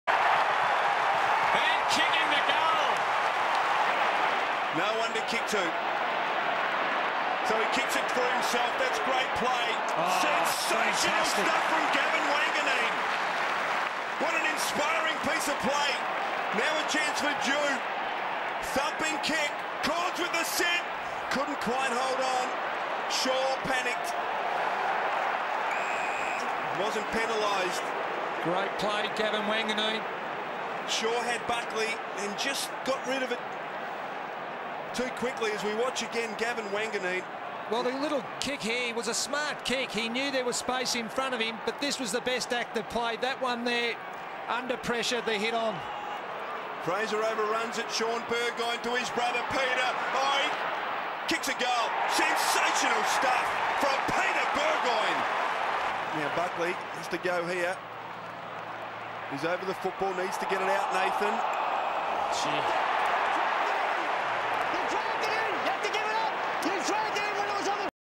Gavin Wanganeen MAGIC Port Adelaide VS Collingwood Peter Burgoyne
Gavin Wanganeen shows off his MAGIC while playing for the Port Adelaide Power VS Collingwood in the 2003 Preliminary Final at the MCG.  Peter Burgoyne then kicks a great goal for the Port Adelaide Football Club.